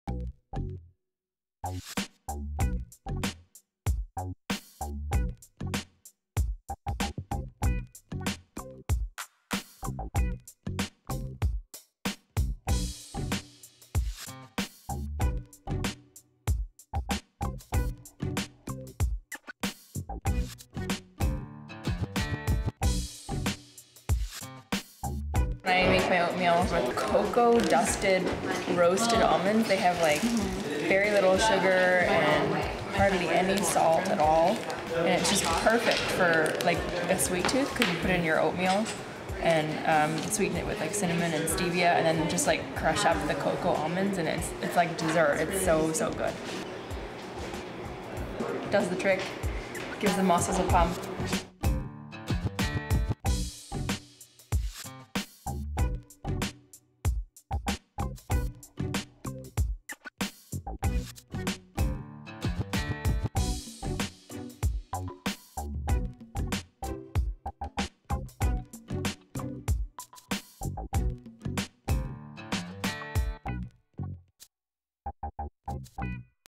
Angelike Psoinos On-Set Nutrition Strategies.mov
During Oxygen's body-weight workout shoot, October 2010 cover girl Angelike Psoinos took time out to share how she keeps her energy soaring. Watch the clip to learn more about Angelike's on set nutrition strategies.